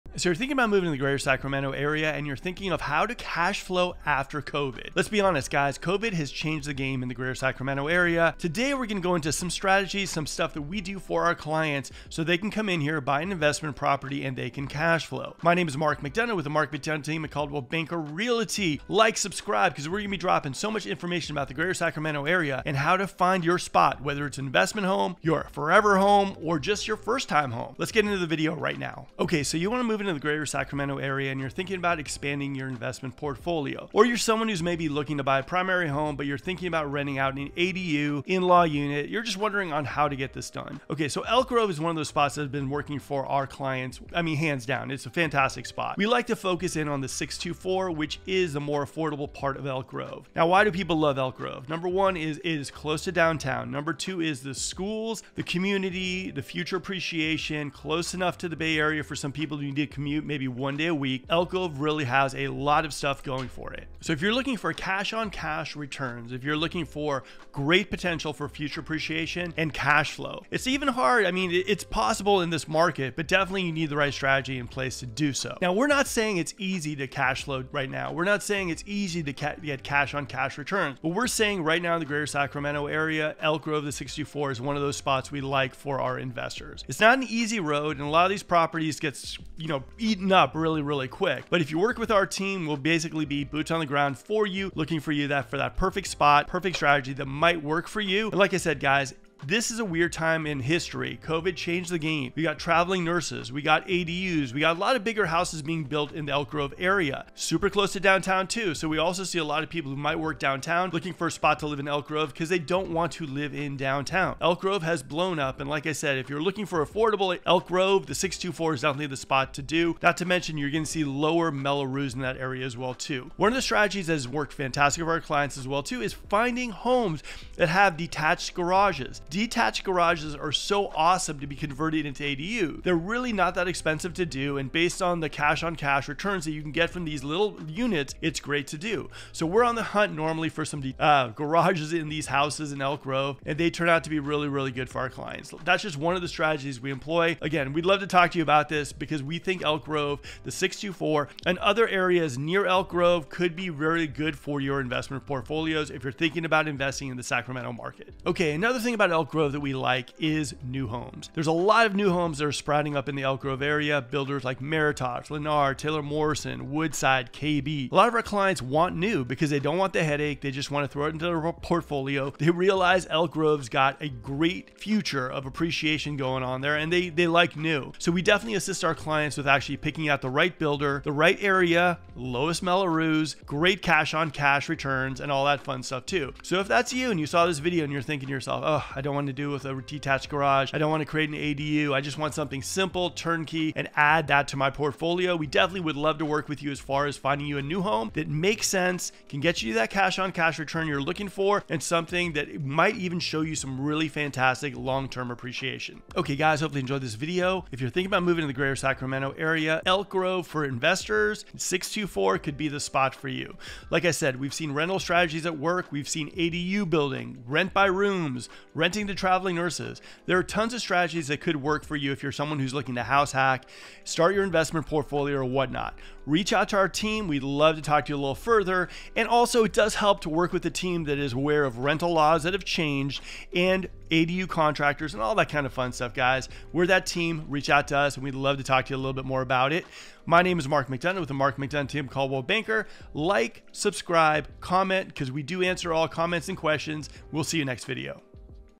🔥 WHY PEOPLE ARE BUYING INVESTMENT HOMES IN ELK GROVE CALIFORNIA | SACRAMENTO HOUSING MARKET NEWS
Why people are buying investment homes in Elk Grove California? Are you moving to California, or are you looking for a place where you can invest in California real estate? Elk Grove Real Estate may be the best option for you. With benefits like being close to downtown, top-rated schools, community and higher chances of future appreciation, Elk Grove California Real Estate could be one of the best investments you could make in 2024. In this video, we will discuss strategies to invest in Elk Grove, California, Real Estate and give you some tips when investing in real estate. Like this content?

[TIMESTAMPS]
00:00 Why INVEST in ELK GROVE in 2024?
00:29 Investing in the Sacramento Real Estate Market
00:40 Cheap Homes for Sale in Elk Grove
00:50 Why People Move to Elk Grove CA
01:15 Investment Properties in Elk Grove CA
02:03 How to be a Real Estate Investor in Sacramento Real Estate
02:38 New Homes in Elk Grove CA
02:56 How to Invest in Elk Grove Real Estate
03:25 Got questions? Let's talk!
03:32 FREE RELOCATION GUIDE TO SACRAMENTO CA
03:42 FREE FIRST TIME HOMEBUYER GUIDE

✅Check out the Latest Houses to Hit the MLS Right Here!

Mark McDonough
DRE#02016066
Coldwell Banker